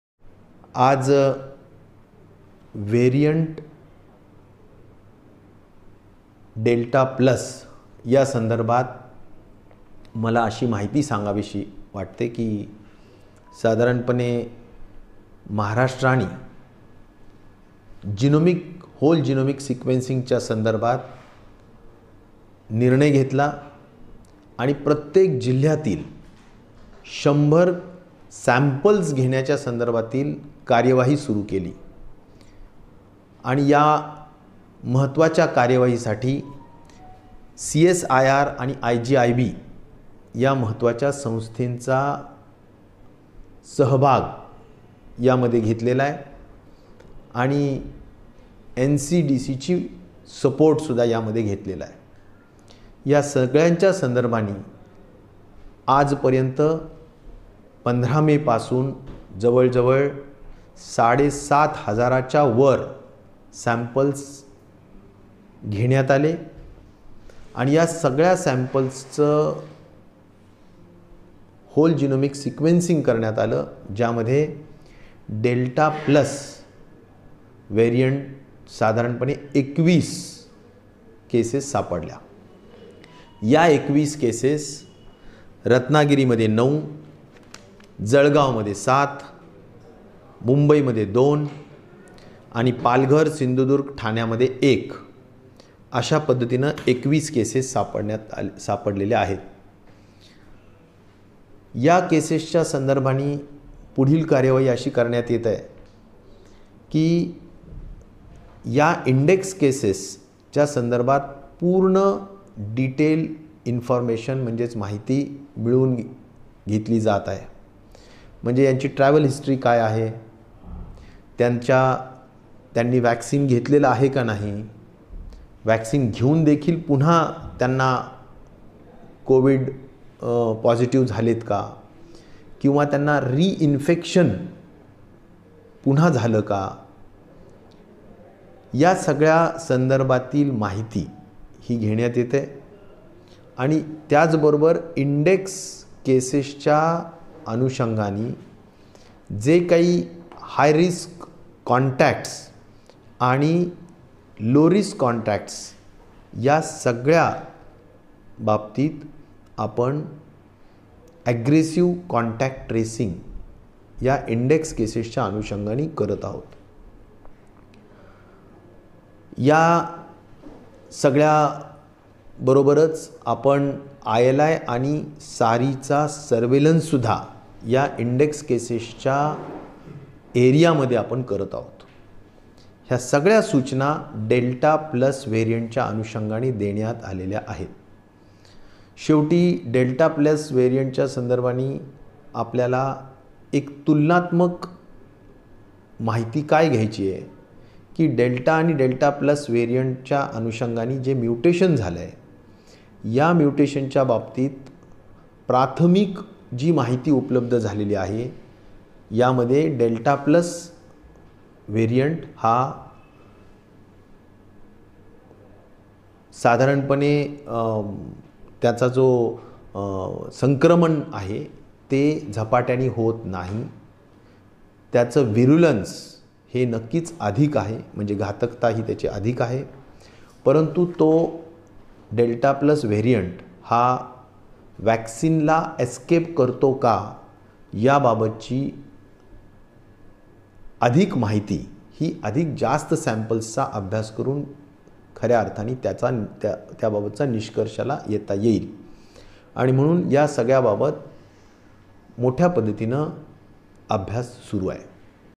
कोरोनाच्या 'डेल्टा व्हेरिएंट +' प्रकाराचे 9 रुग्ण रत्नागिरीत सापडले सिंधुदुर्गात 1 : आरोग्यमंत्री
delta_plus_variant
rajesh tope
sindhudurg
ratnagiri